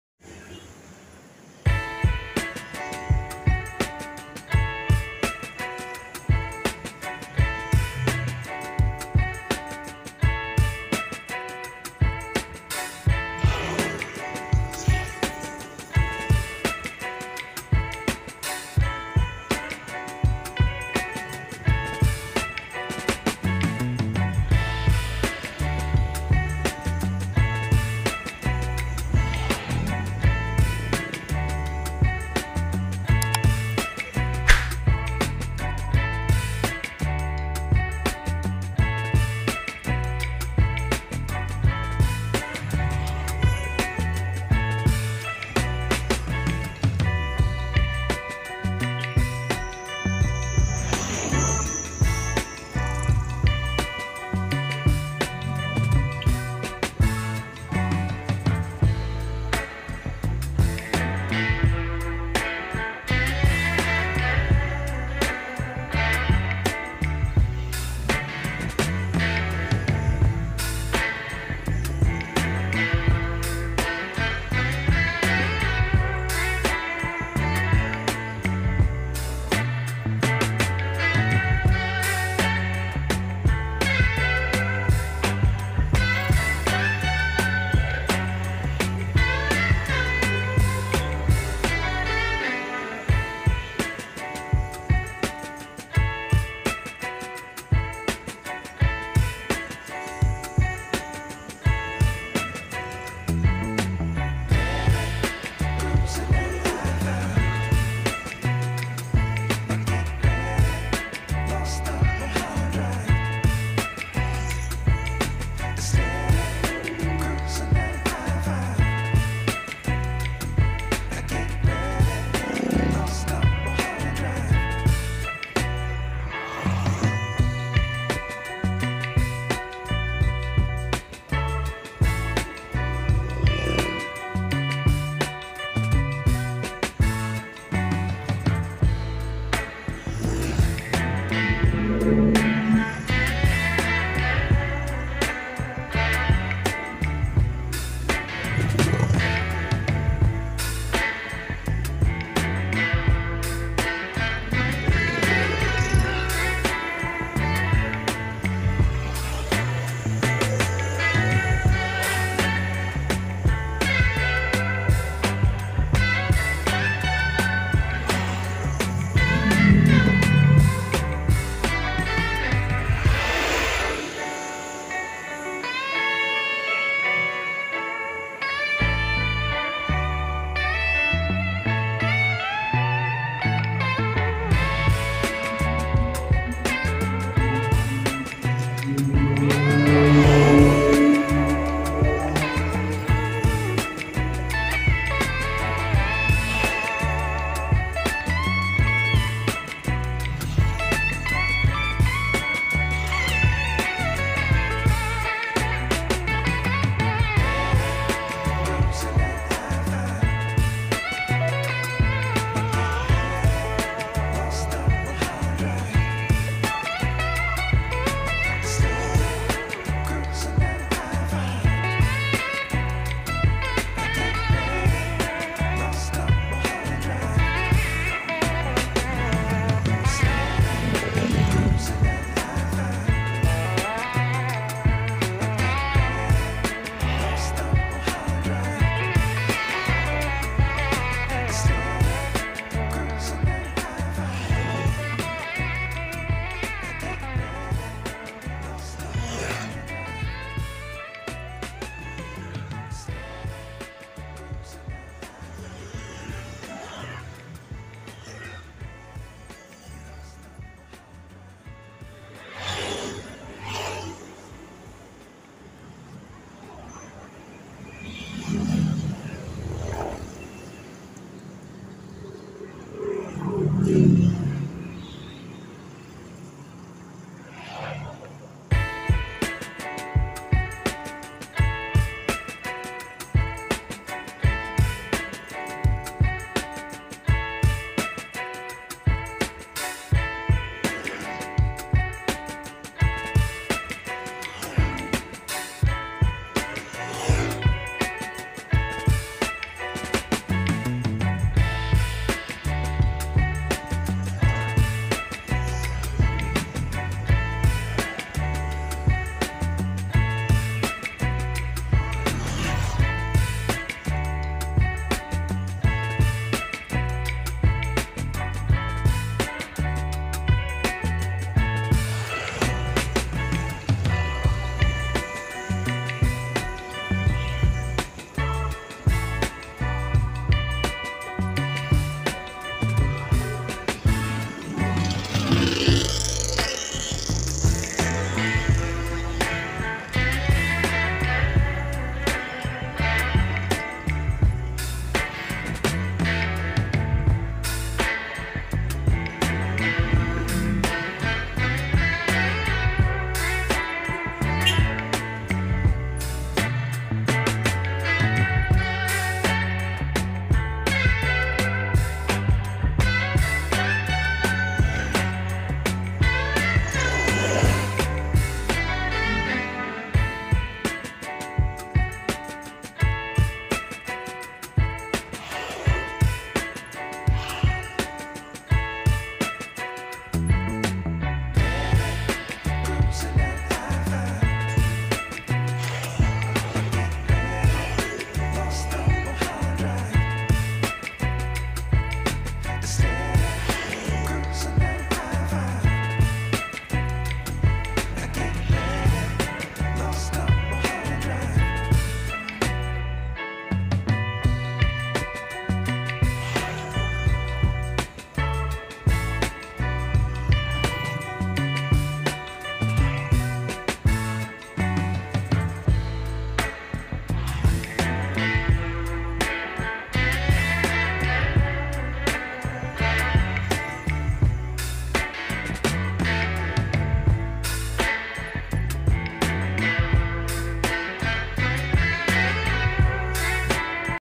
Gowes sepeda | Jl Imogiri Barat, Ngotho, melewati Pendapa Tulungo milik Soimah
Assalamualaikum wrwb.

Wassalamualaikum wrwb.